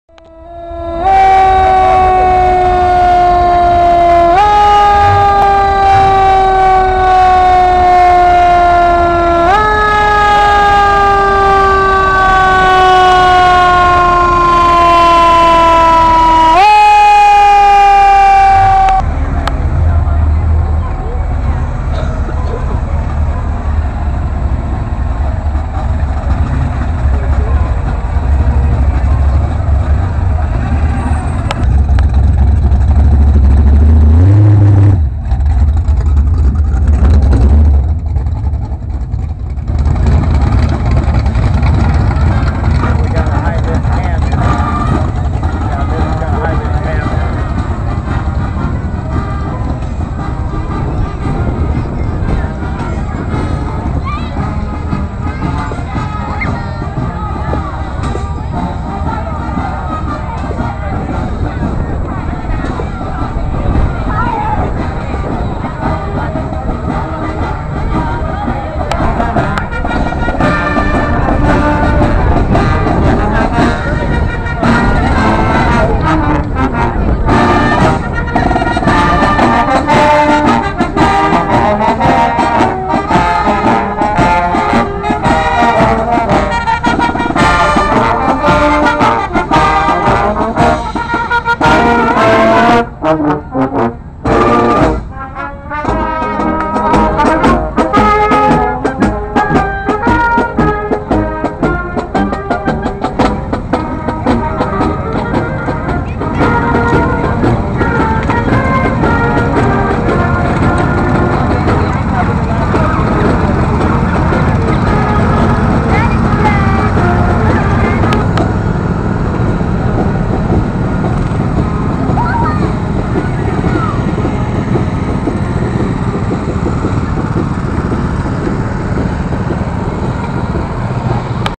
Mt.Zion, IL Neighborhood Parade
Each Independence Day neighbors in Mt. Zion celebrate with a childrens' parade including a band, clowns, popsicles and watermellon.  Listen for information about Mt. Zion happenings on WXFM, 99.3.